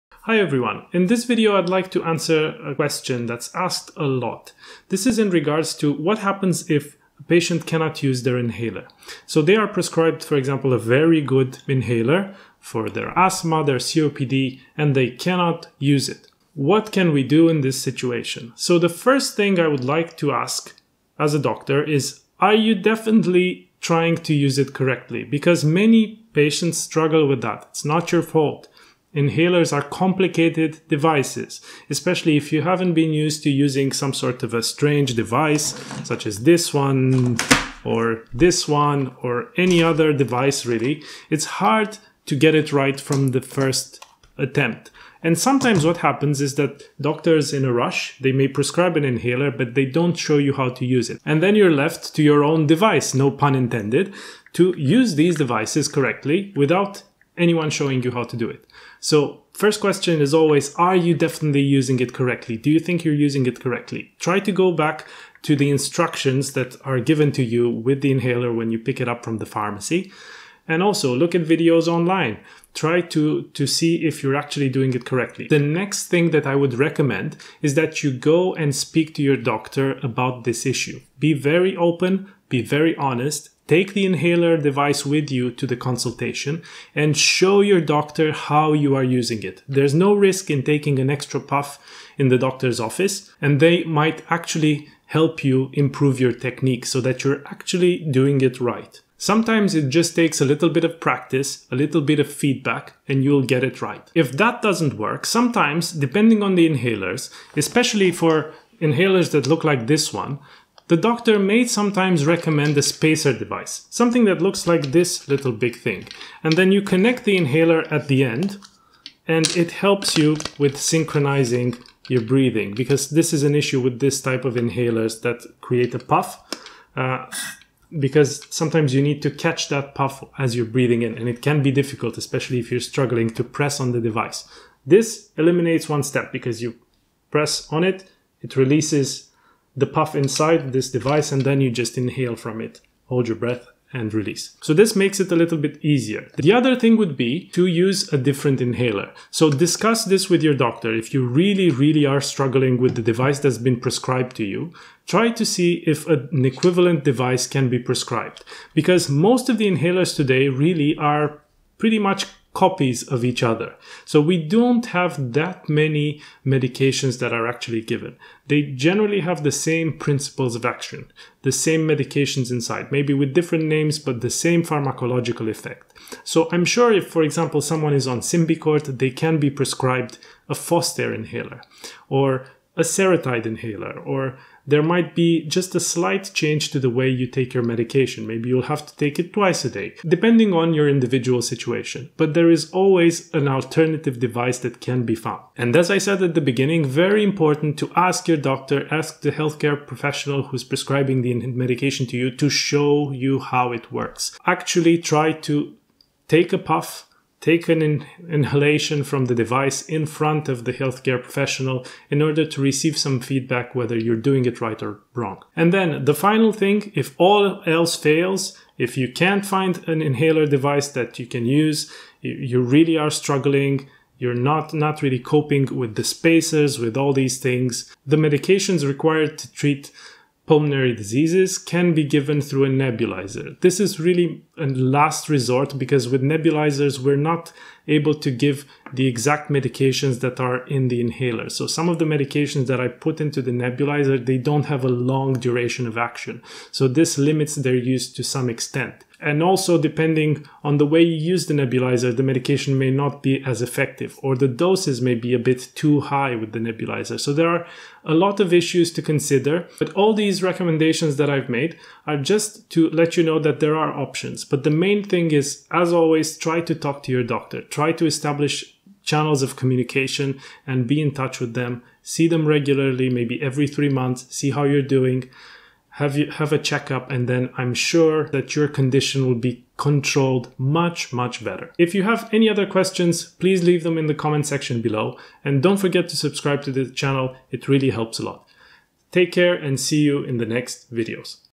Can't use inhaler - what to do?
In this video some suggestions on what to do if you cannot use the inhaler prescribed by your doctor. Inhaler technique can be difficult and sometimes you may just have to keep practicing while receiving feedback from a health professional. There are also alternative inhalers available, or you may need a spacer device. Sometimes a nebuliser can be also used.

✔️Videos on my channel are free to watch, but your support is very appreciated! A "virtual" coffee (latte, espresso or even decaf) helps a lot☕